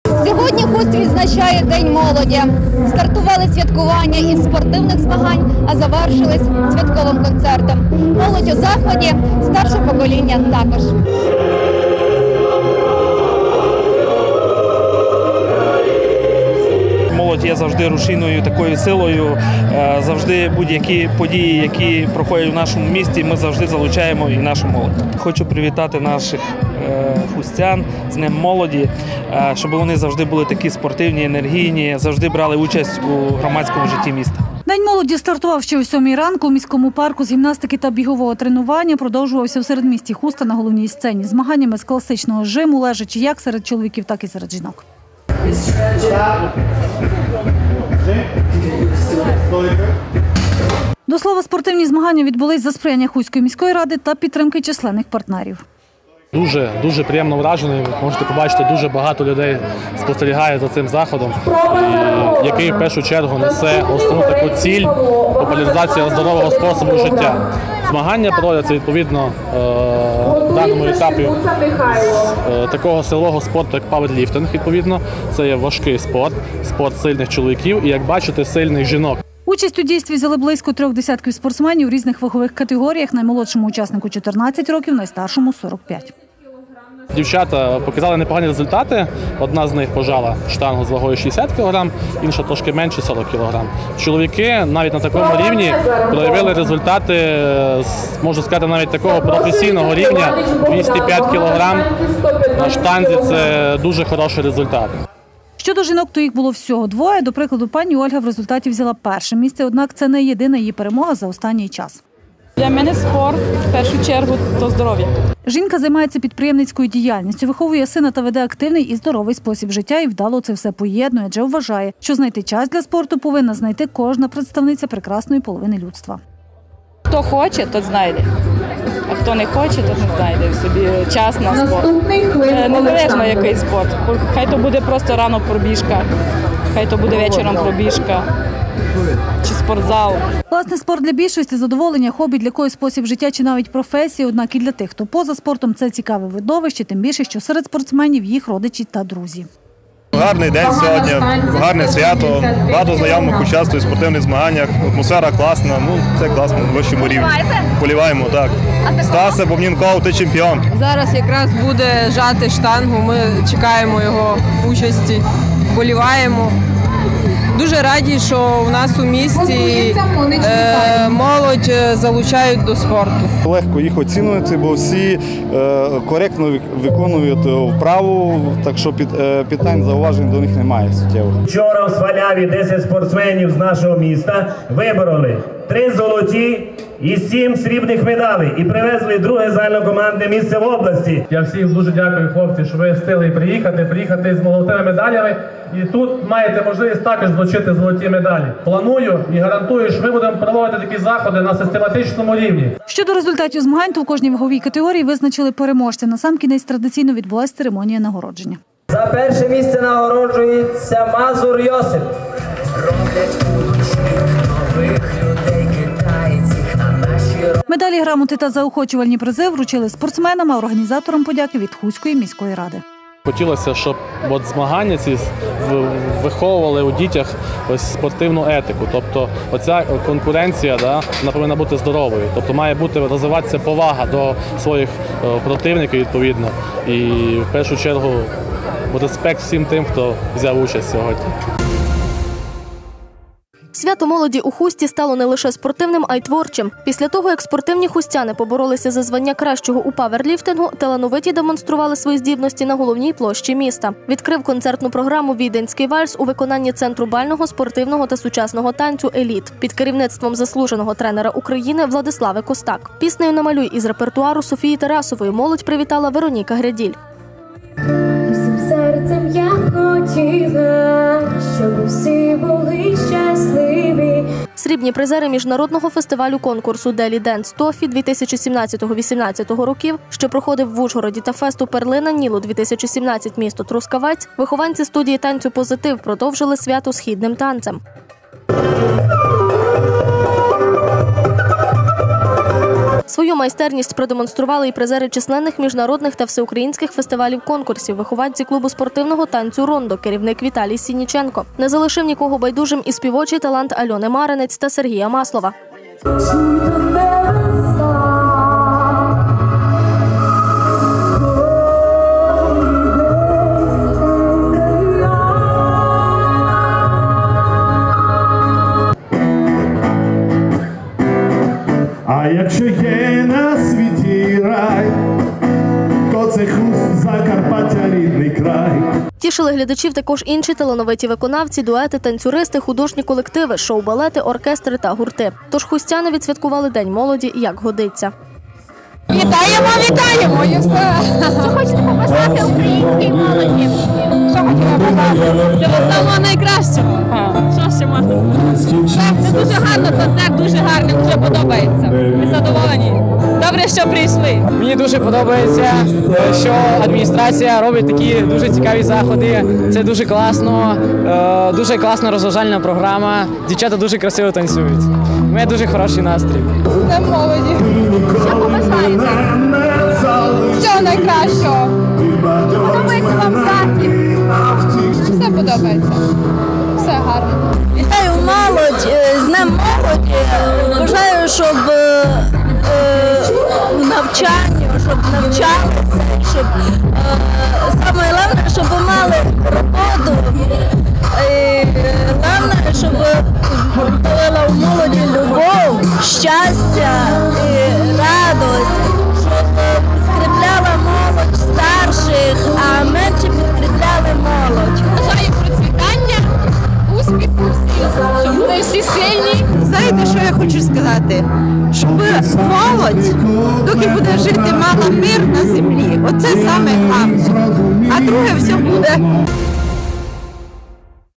Свято молоді у Хусті
День молоді стартував ще о сьомій ранку у міському парку із гімнастики та бігового тренування. А продовжилось свято у середмісті Хуста на головній сцені – змаганнями з класичного жиму лежачи як серед чоловіків, так і серед жінок.